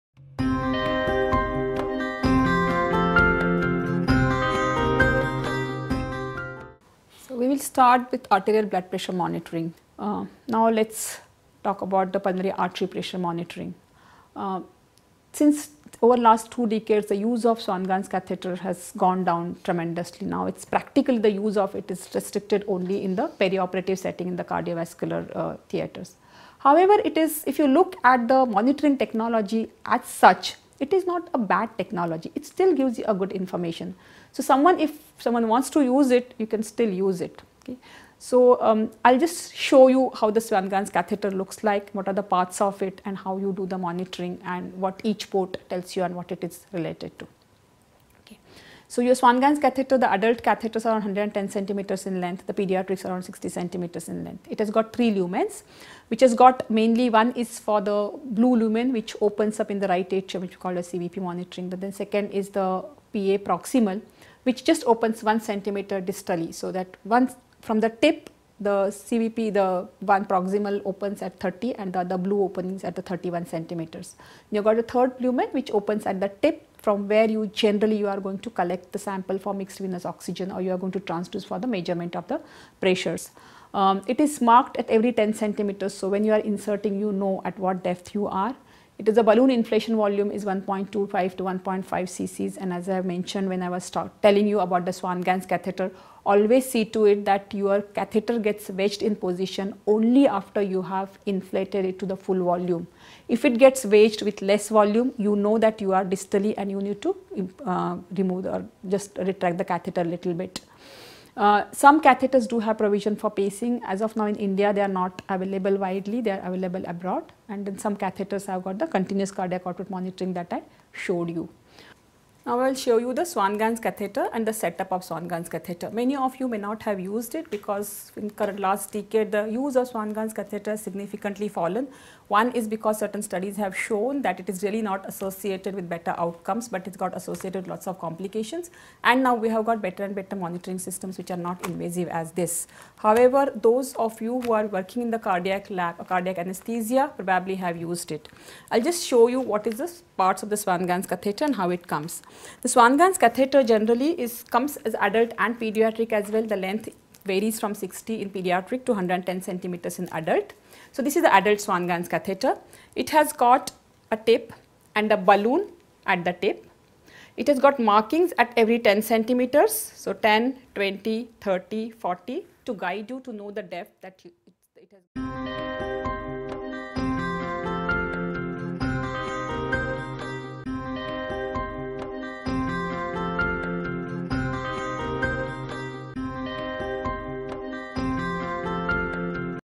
Workshop - Advance Hemodynamic - Dr Vijaya Patil - 4C (Comprehensive Critical Care eCourse)
ISCCM's - 4C (Comprehensive Critical Care eCourse)
[NBE Accredited]

- An Introduction by Dr Rajesh Chawla
- A comprehensive 40 modules e-Course on the important aspects of critical care. Accredited by National Board of Examinations (II-A).